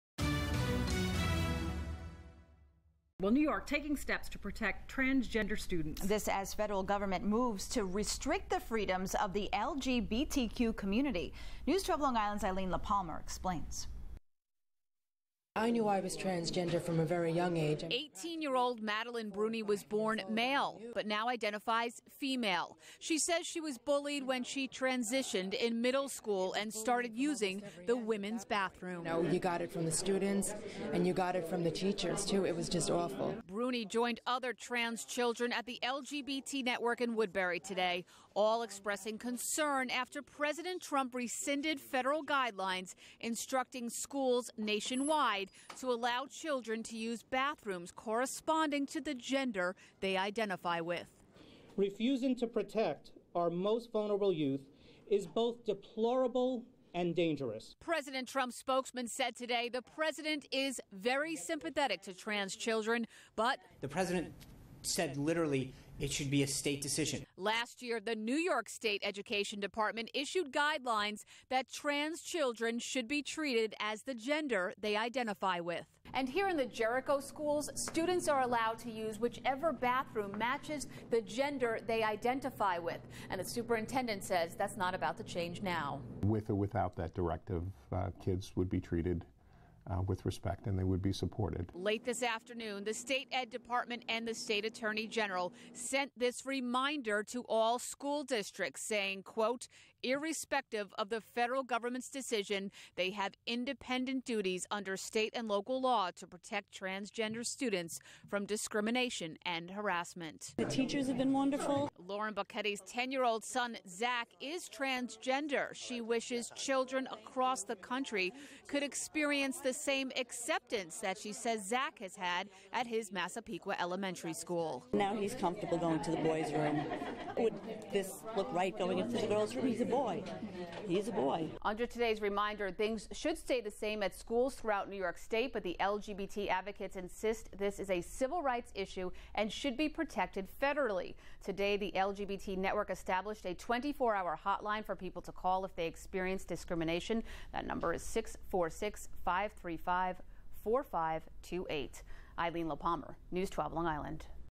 News12 | NY AG, Education Dept reaffirm support for transgender students
LGBT Network CEO David Kilmnick hosts a press conference to uplift transgender youth and speak out against President Trump's attack on the rights of transgender students and the LGBT community.